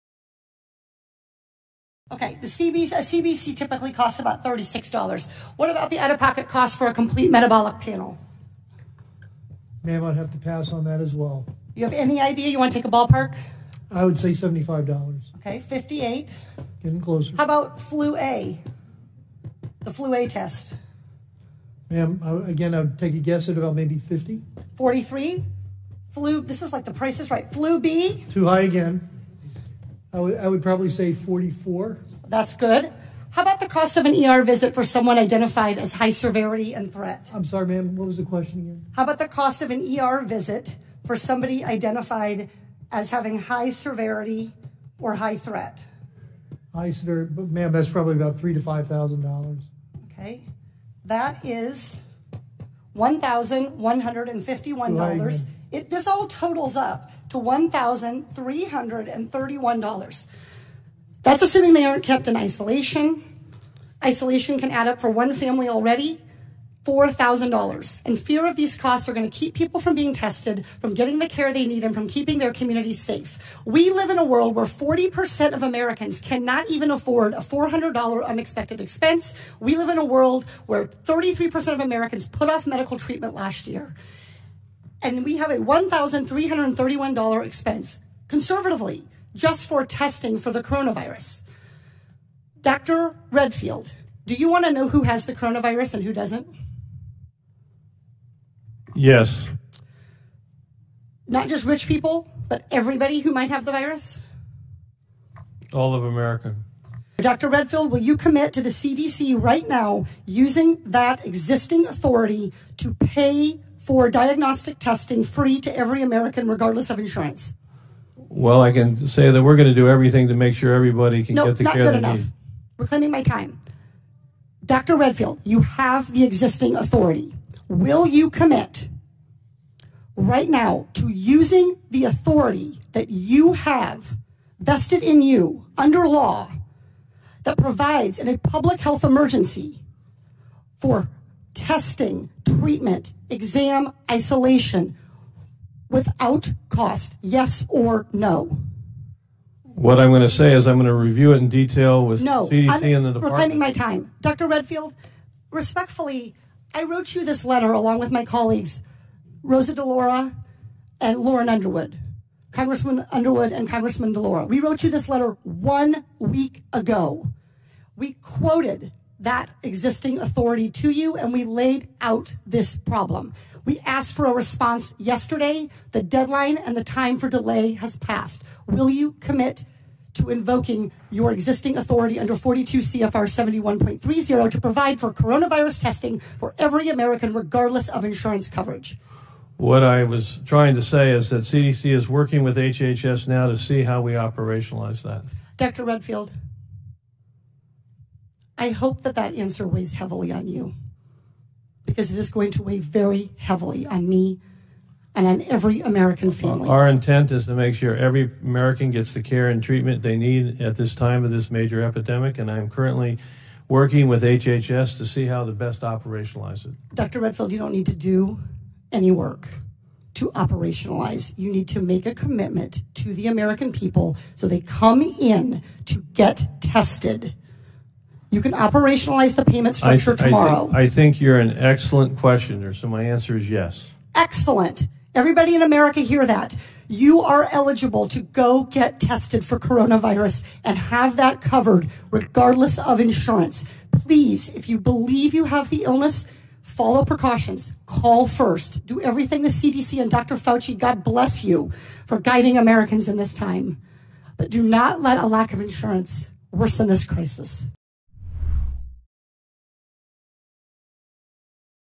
ส.ส.สหรัฐฯอภิปรายขอให้ตรวจ COVID-19 ได้ฟรี l Workpoint Today
เกาะติดทุกข่าวที่น่าสนใจ กับข่าวเวิร์คพอยท์
การเมือง สังคม เศรษฐกิจ ต่างประเทศ นโยบายสาธารณะ สุขภาพ คมนาคม

Podcast by Workpoint News